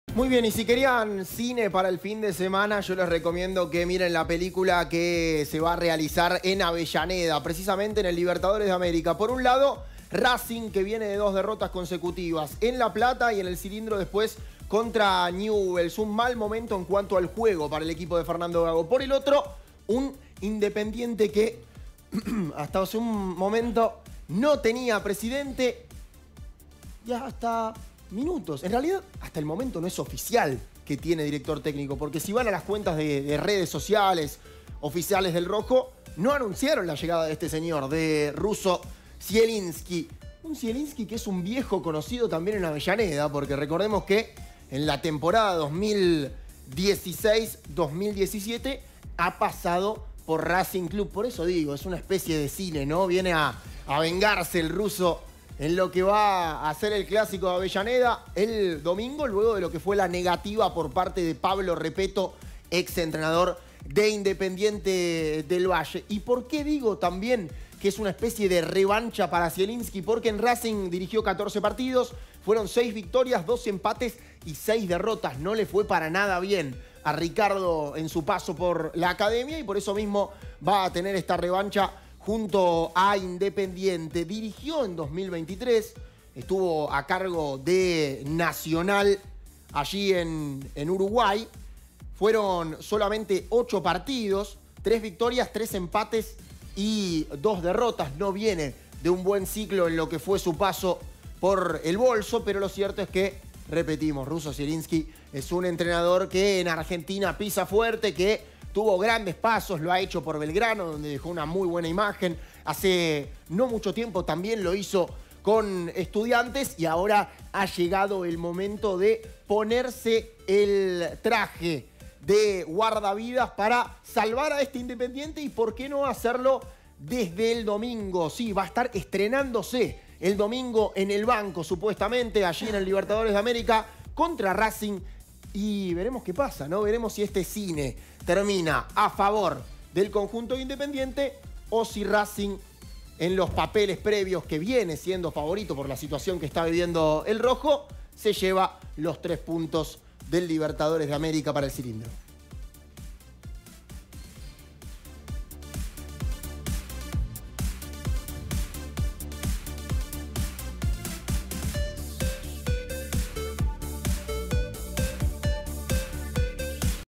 Ricardo Zielinski llegó a un acuerdo para dirigir a Independiente tras la negativa de Pablo Repetto
👉 El director técnico de 63 años acordó de palabra y firmará su contrato este viernes. El Ruso debutaría en el banco del Rojo en el clásico ante la Academia por la Liga Profesional.